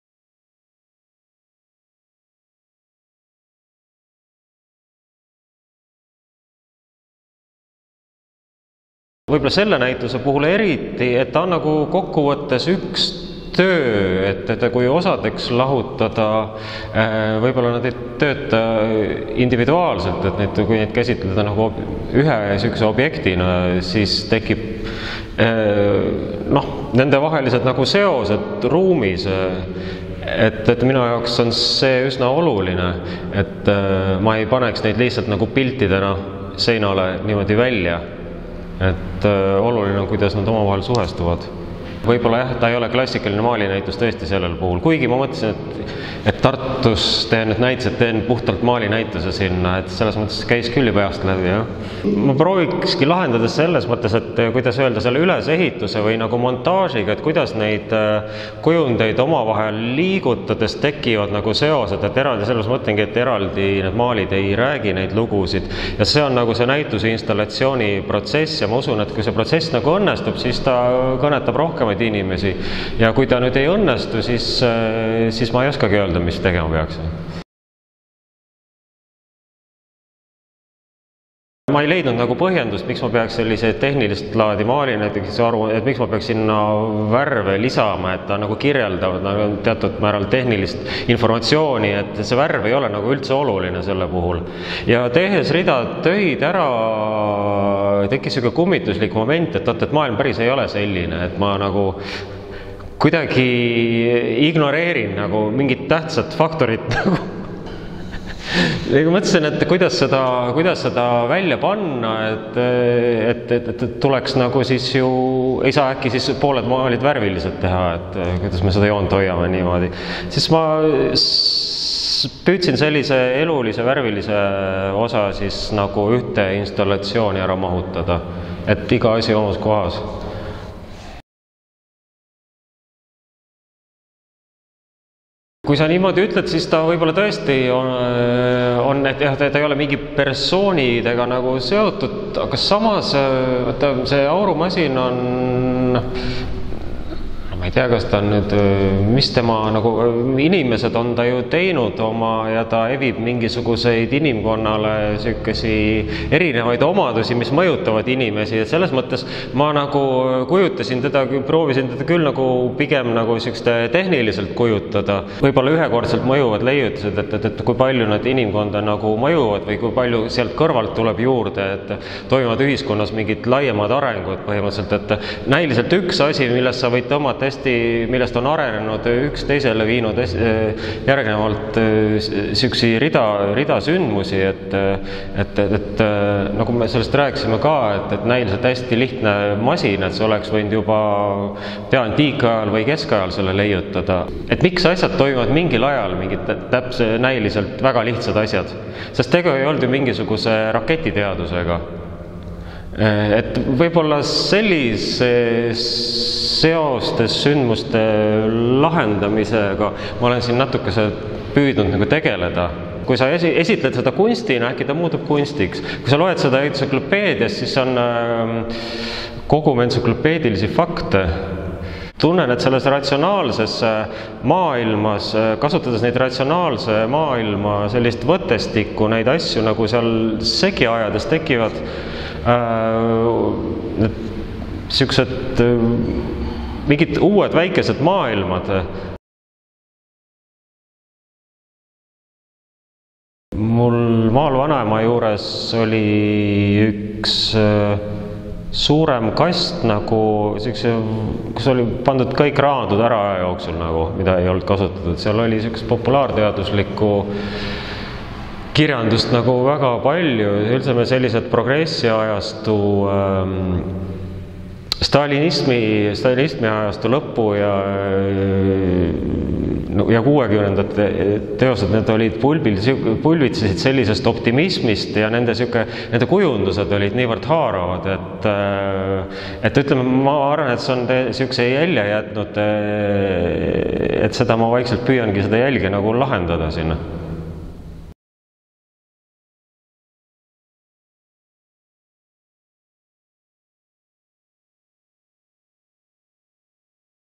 Holger Loodus "Volüüm. Imaginaarne kontsert seitsmele aurikule"
Holger Loodus räägib oma Tartu Kunstimaja väikese saali näitusest "Volüüm. Imaginaarne kontsert seitsmele aurikule" 10.03.–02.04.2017.

Holger Looduse värske maaliseeria „Volüüm. Imaginaarne kontsert seitsmele aurikule” tegeleb kunstniku sõnul modernismi sünni lahtimõtestamisega. Aurumasin, käesoleva näituse keskne figuur, näib küll abstraktsem ja vähem jutustav kui kunstniku senised maaliseeriad, mis on pajatanud nii tuleviku inimestest, koeraga jalutamisest kui puhkusele sõitmisest. Ent tuleb tõdeda, et kui esialgu leidsid kineetilised objektid tee maalikunstniku töövahendite arsenali, siis nüüdseks on nad hiilivalt asunud okupeerima ka maalide sisu. Kummastav on aga sealjuures asjaolu, et maalide jutustavus ja ennekõike filmilikkus, mille läbi Loodust tuntakse, on seeläbi veelgi kasvanud.

Holger Loodus (s. 1970) lõpetas 2008. aastal Eesti Kunstiakadeemia muinsuskaitse ja restaureerimise osakonna, täiendas end 2011. aastal Karlsruhe Disainiülikooli meediakunsti osakonnas ja lõpetas 2012. aastal Eesti Kunstiakadeemia magistriõppe maali erialal. 2012. aastal pälvis Holger Loodus isiknäitusega "2150. Portreed" Draakoni galeriis Noore Kunstniku preemia. Lisaks Tallinnale on Loodus esinenud Berliinis ning osalenud grupinäitustel Tallinna Kunstihoones ja Kumus.